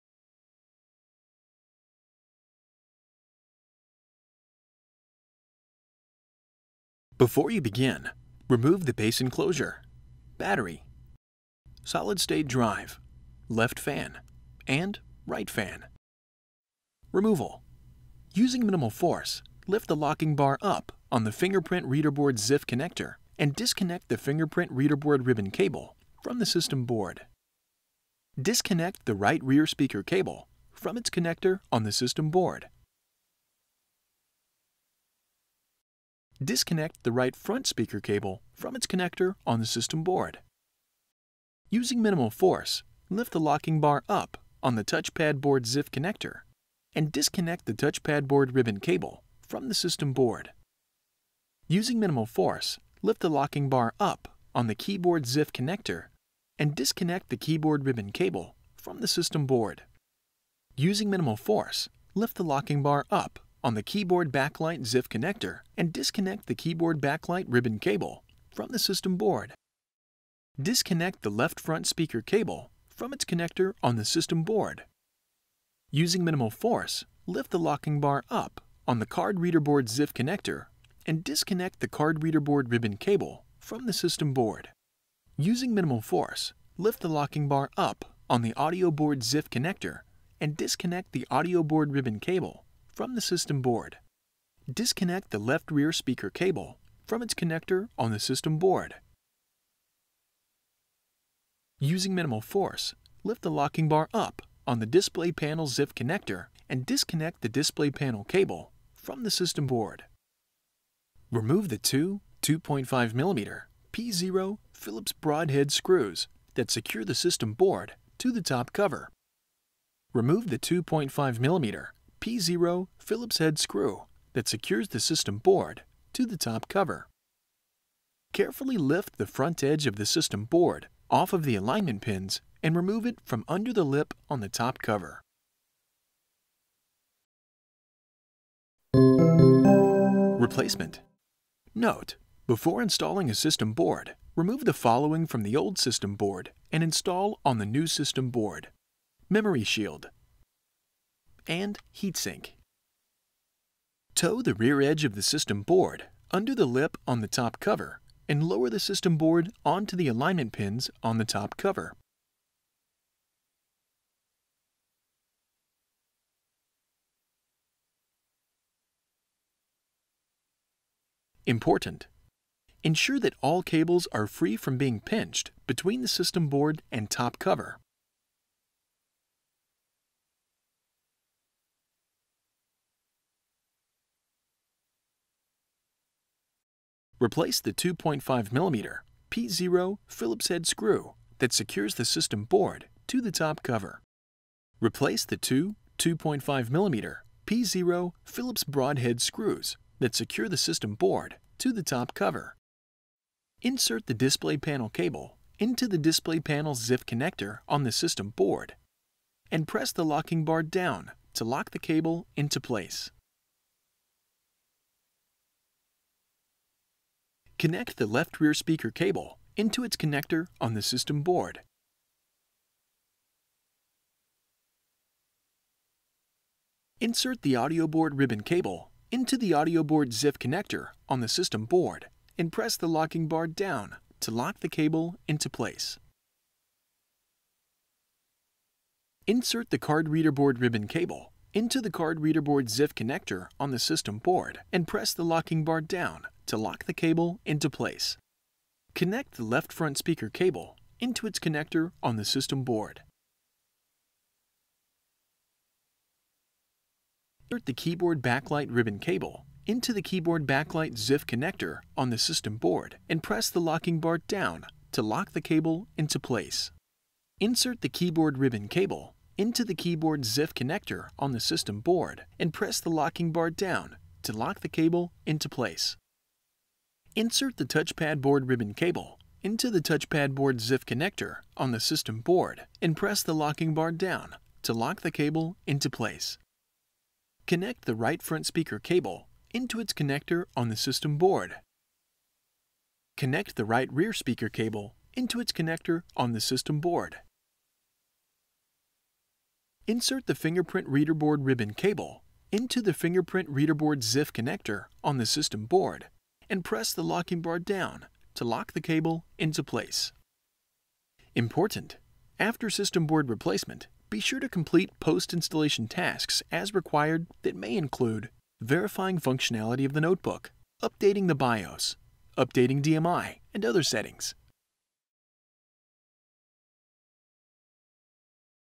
Remove and Replace the System Board | HP Spectre x360 13
Learn how to remove and replace the system board on the HP Spectre x360 13.